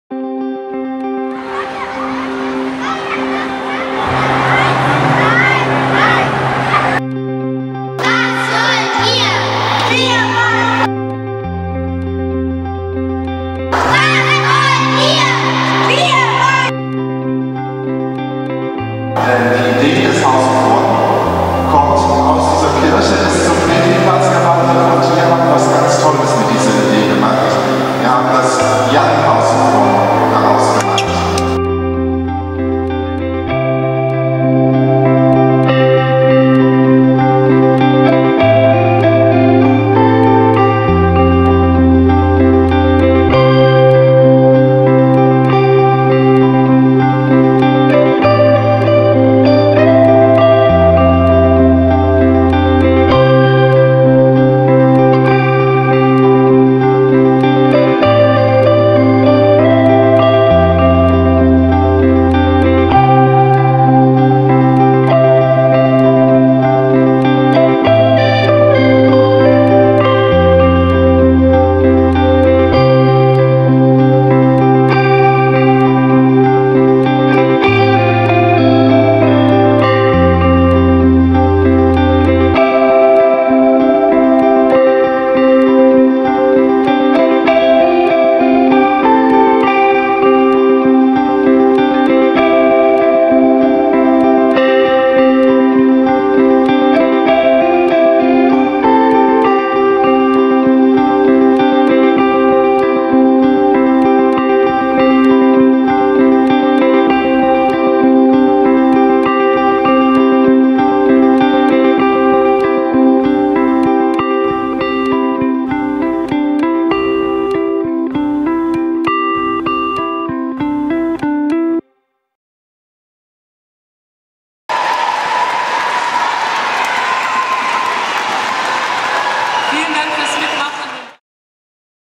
Young House of One - Abschlusspräsentation in der St. Marienkirche
Am 20. Oktober 2017 fand die Abschlusspräsentation des Projektes Young House of One in der St. Marienkirche in Berlin statt.
Das gemeinsame Projekt von ALICE - Museum für Kinder im FEZ und dem House of One hatte im Sommer 2017 unter anderem auf dem Berliner Petriplatz stattgefunden. Zum Abschluss dieses Projektes kamen viele Kinder und Jugendliche aus den beiteiligten Schulen und Gruppen nach Berlin.

Martin Luther und die Wirkung seiner Thesen sind in der Gegenwart auch hiermit angekommen.